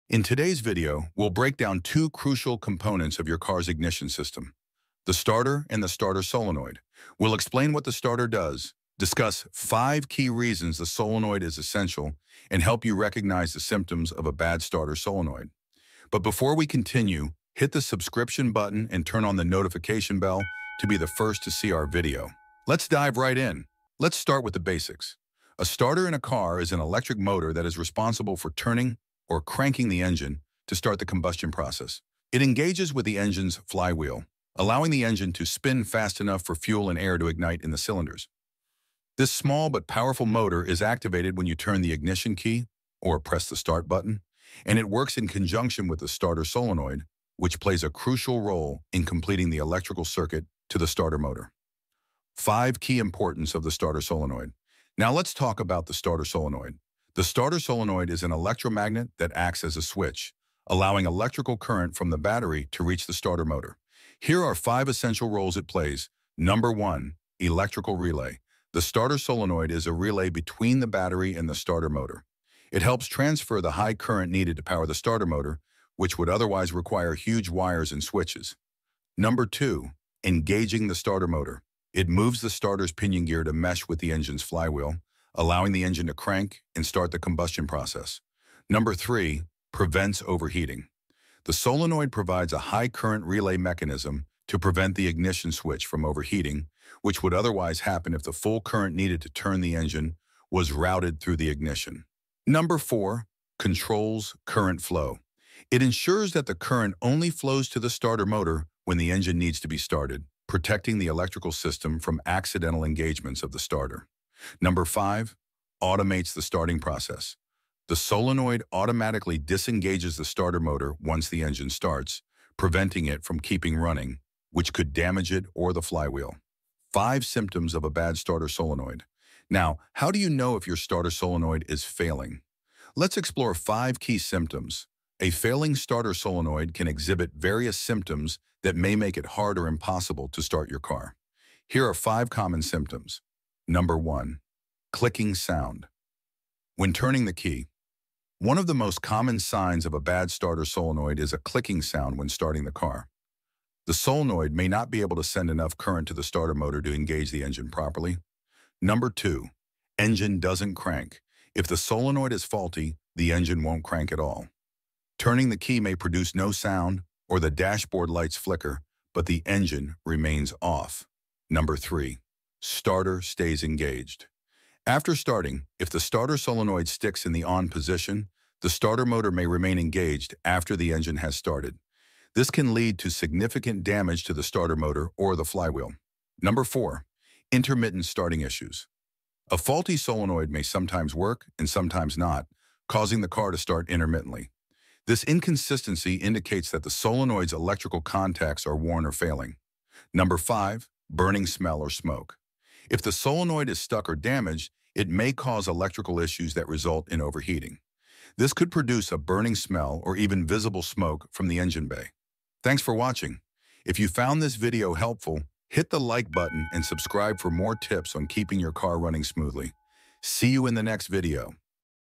How to Check Your Car Kick Starter and Solenoid for Common Issues
In this video, we’ll show you how to check your car starter and solenoid for common issues that could prevent your engine from starting. Learn the basic functions of these components, how to diagnose problems like a faulty starter solenoid, and what symptoms to watch for. Whether you're a car enthusiast or want to avoid being stranded, this guide will walk you through checking and identifying potential starter and solenoid problems.